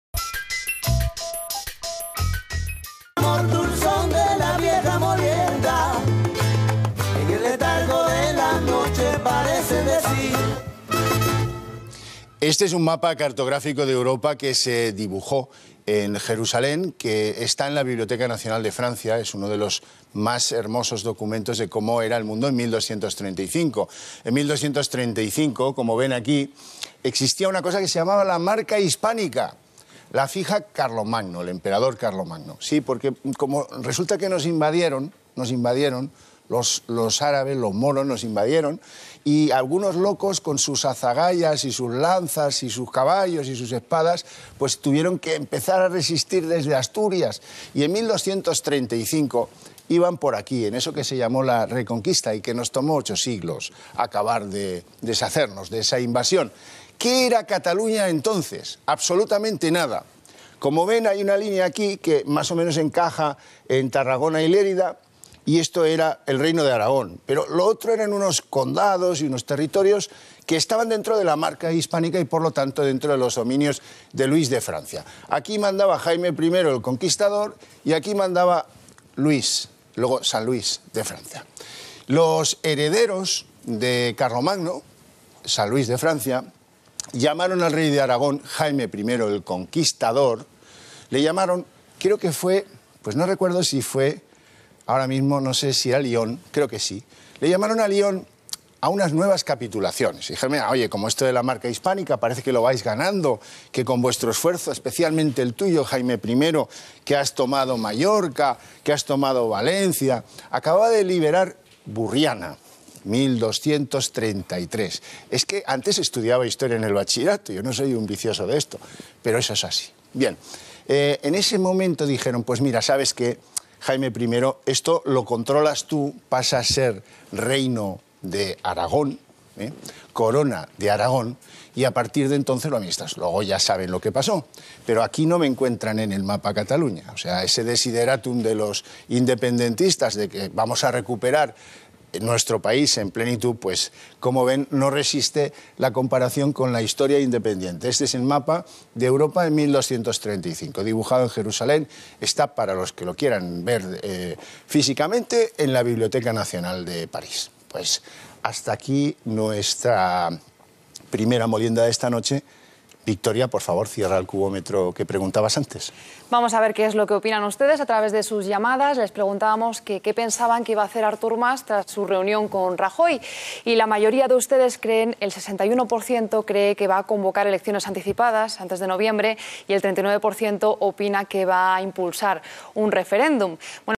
En 1235 cataluña no existía, pero si los Reinos de Aragón, Valéncia y Mallorca
Europa de 1235, la cual ratifica la autenticidad del Tractat de Corbeil de 1258, donde la "Marca Hispánica" hasta ese año 58 perteneció a território Francés.

Por lo que aplasta cualquier teoria de repoblación por parte de Catalanes, ya que durante la reconquista del Reino de Valéncia, éste territorio no pertenecía en absoluto a la Corona de Aragon, sinó que era parte feudataria del Reino de Francia.